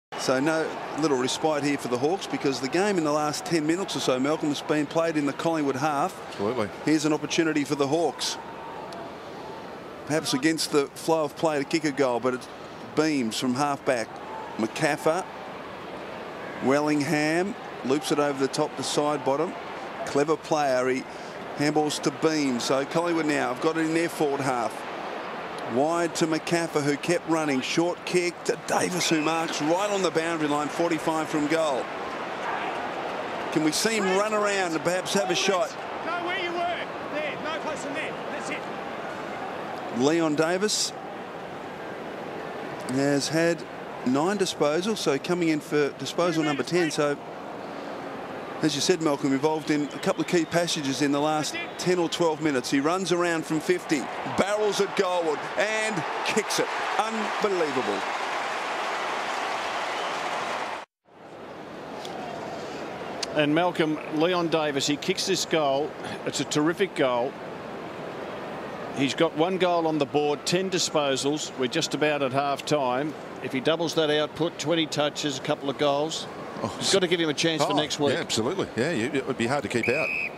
Leon Davis the perfect kick
Leon Davis showing off his perfect goal kicking skills on show at the mcs for the collingwood magpies in their round 22 encounter with the Hawthorn Hawks